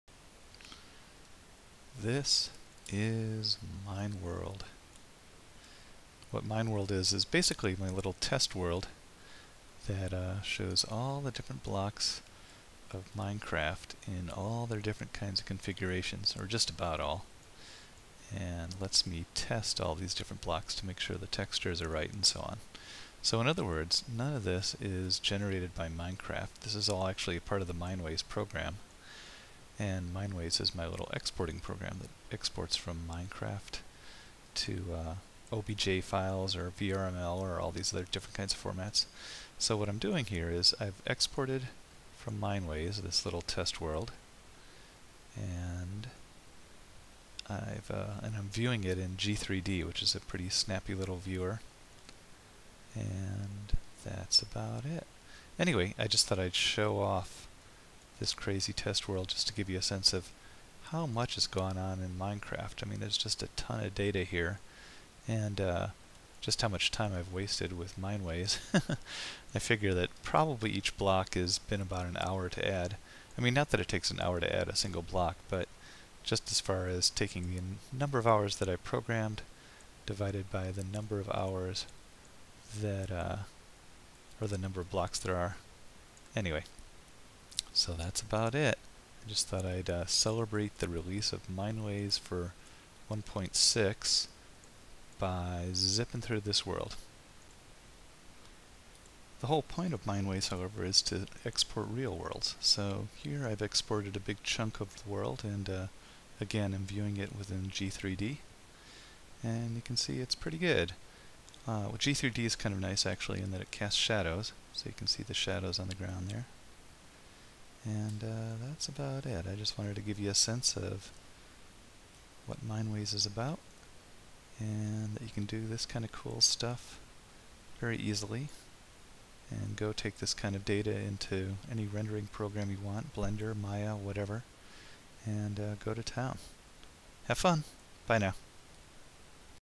Mine World
It's a play on words, get it? Get it?!

I just updated Mineways to support block from Minecraft 1.6. What you're seeing is my testing world for [Mineways] which doesn't exist in Minecraft. Rather, Mineways itself generates this block test world - it's a handy feature, as you can preview any blocks and see how I'll output them. I then cut to the spawn area of [Vokselia]

I'm zipping through it using [the G3D viewer] which works pretty well with Mineways' OBJ files.